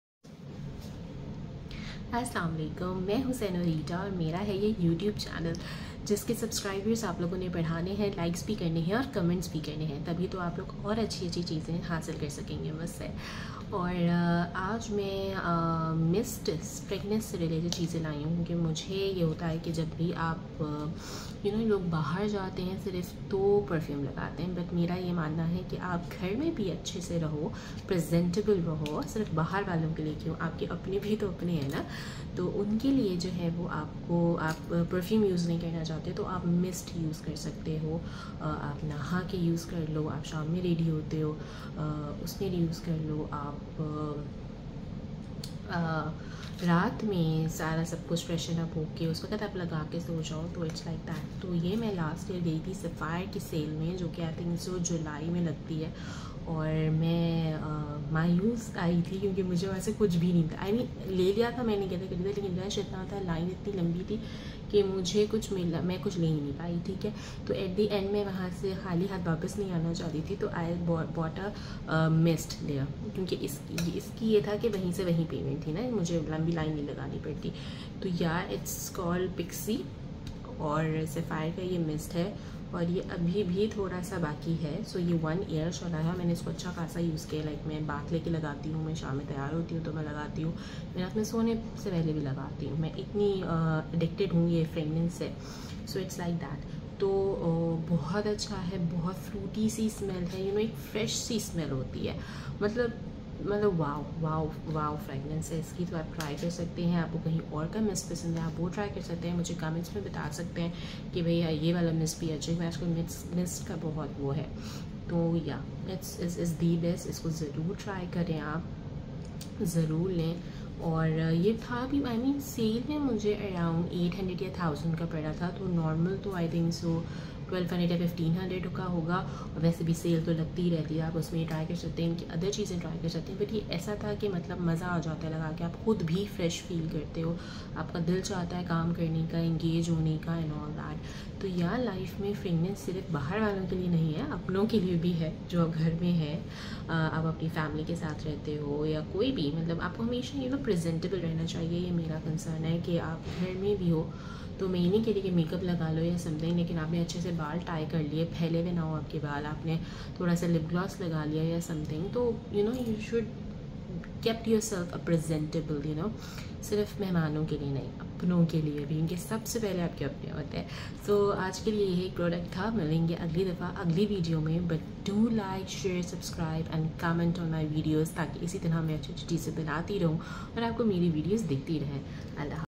Refreshing and Long-Lasting: Sapphire Body Mist Review
Looking for a refreshing and affordable fragrance? This Sapphire body mist is the perfect choice. In this video, I'll share my experience using this delightful scent and why it's become a staple in my beauty routine.
I love the fruity Flory aroma of this body mist. It's light, refreshing, and perfect for everyday wear. The mist is long-lasting, keeping me smelling fresh throughout the day.
If you're looking for a quality body mist that won't break the bank, I highly recommend this Sapphire fragrance. It's a must-have for anyone who loves to smell good.